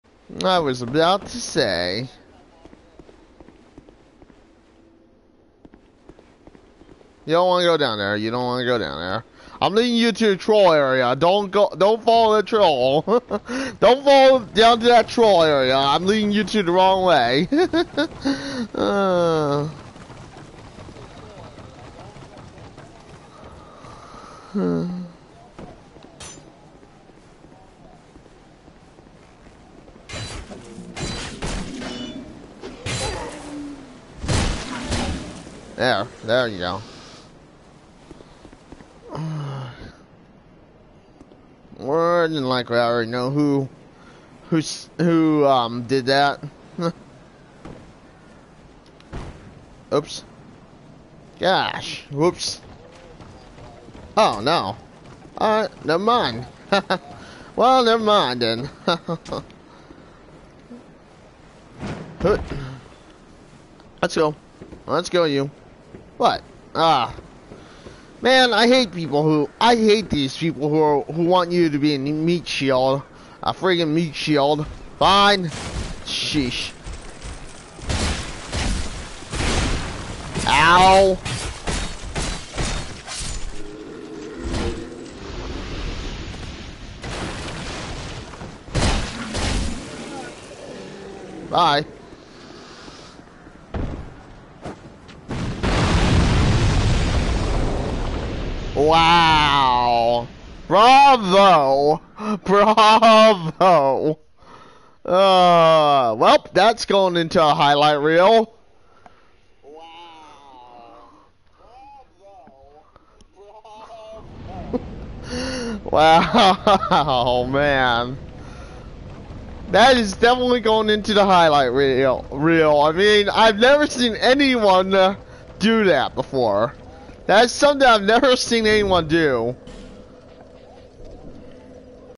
Acting like a Meatshield/ Facepalm Fail
Really ive never seen that happen before Bravo and congrats if you found this video your Epic Fail has now been immortalized as a video hope you enjoyed the laugh like i did when i first saw this -- Watch live